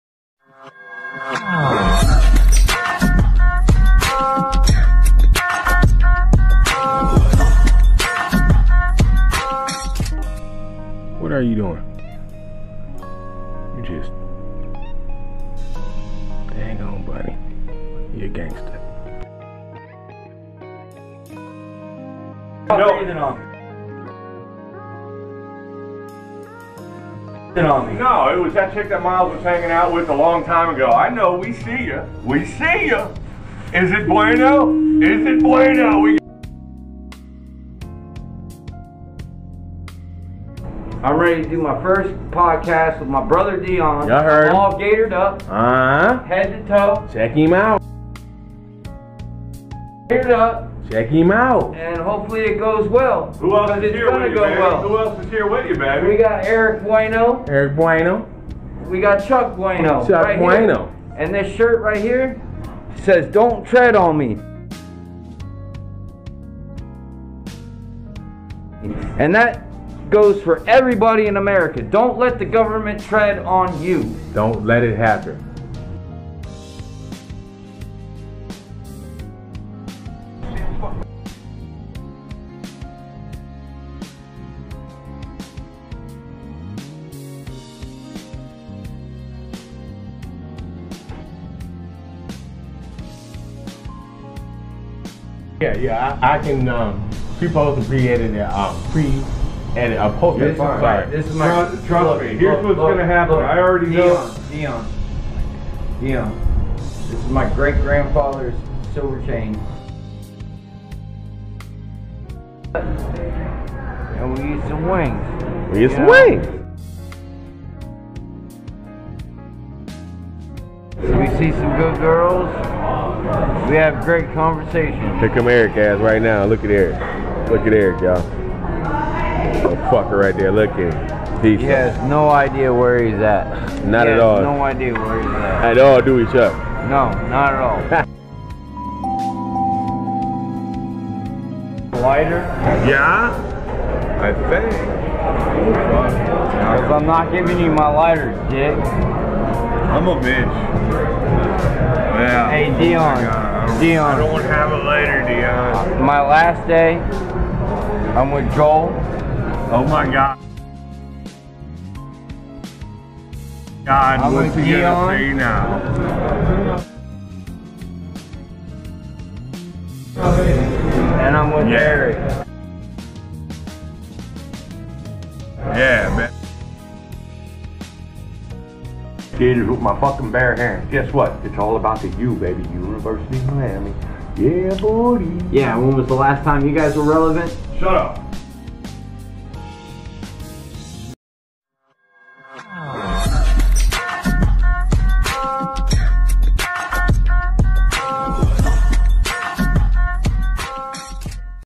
Dott's View Podcast: After hours pt.2
Today's  video: we show you behind the scenes after hours
Show Topic: podcasting and after hours
Show: Dott's View Podcast: After hours pt.2

Today we have Dott's View Podcast: After hours pt.2 is me Chuck and Eric doing our thing. Take a second to leave some love for this after hours. All are welcomed.to the channel.